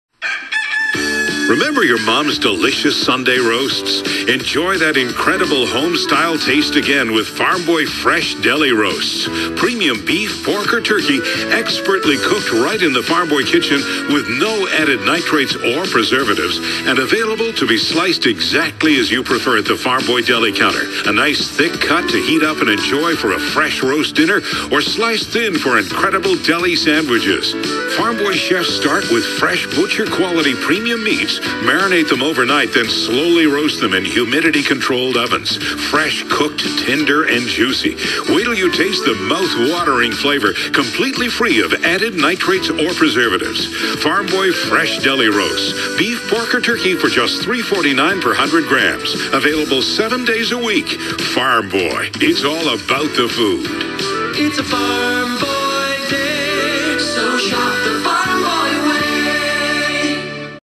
Farm Boy Commercial: Fresh Deli Roasts
Today, I was listening to Majic 100 early. Something tells me when I was writing things down, and probably drew a couple of things that I love, this commercial came on. Here it is for Farm Boy with the fresh deli roasts. No infringement needed. Farm Boy fresh markets own the rights!